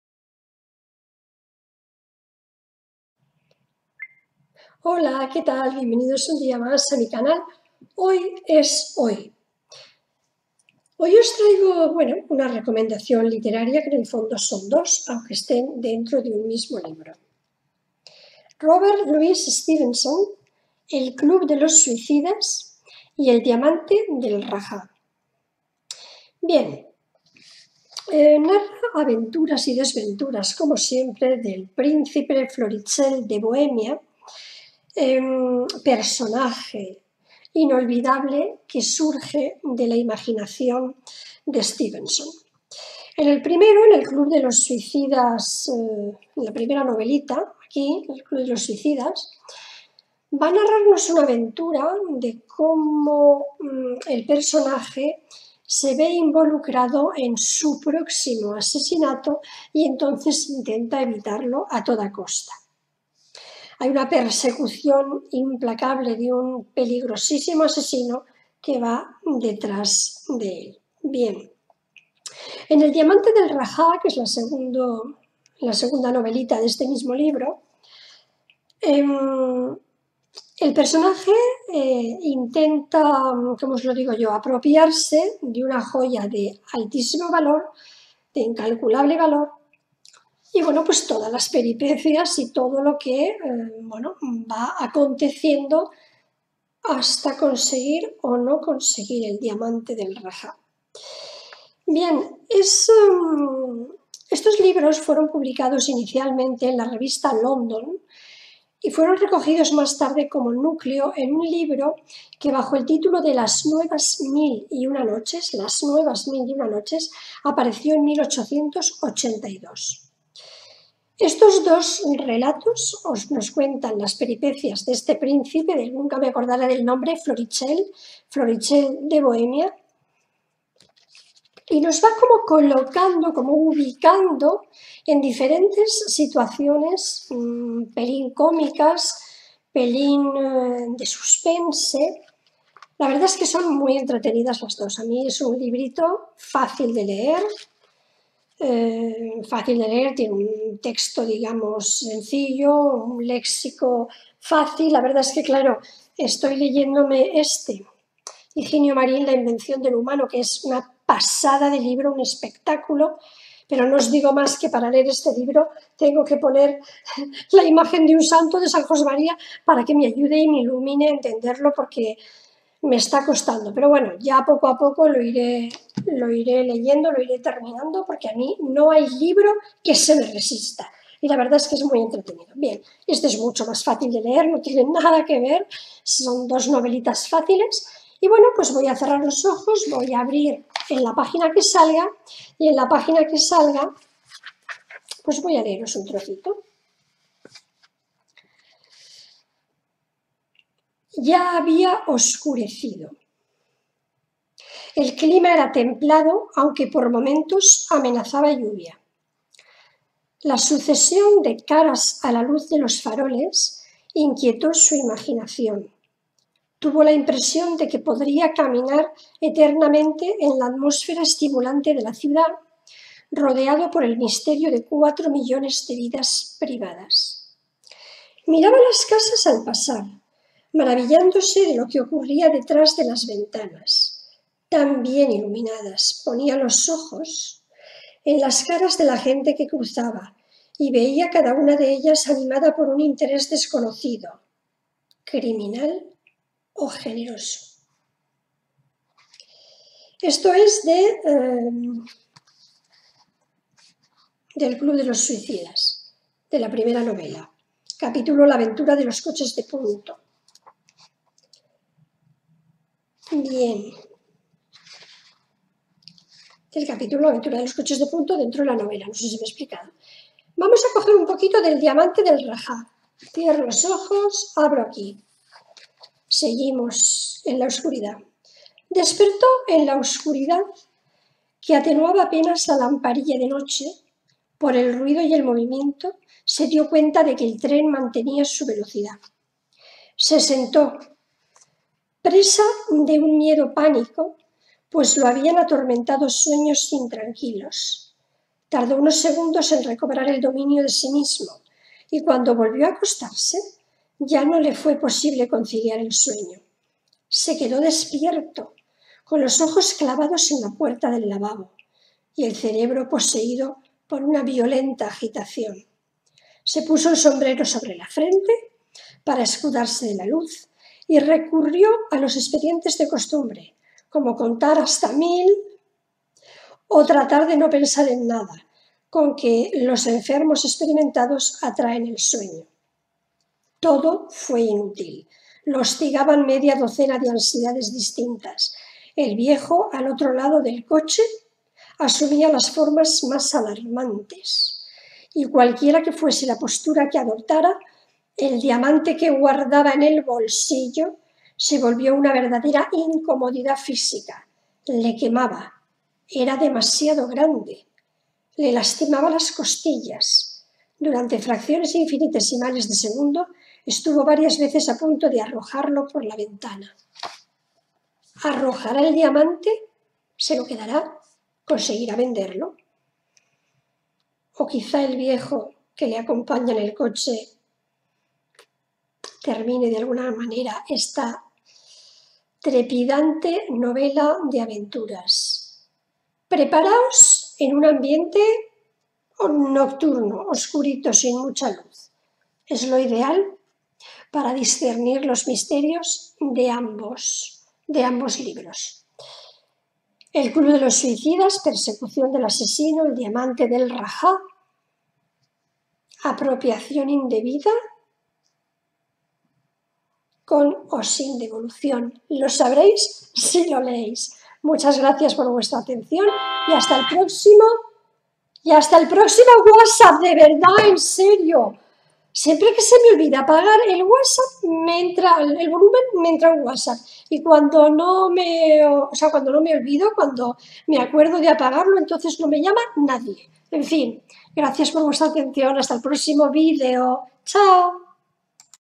DOS NOVELAS, PARTE de las " NEW ARABIAN NIGHTS "y LA INVENCIÓN DE LO HUMANO de HIGINIO MARÍN PEDREÑO
Robert Louis Stevenson nos introduce en dos mundos diferentes de aventuras que no te esperas...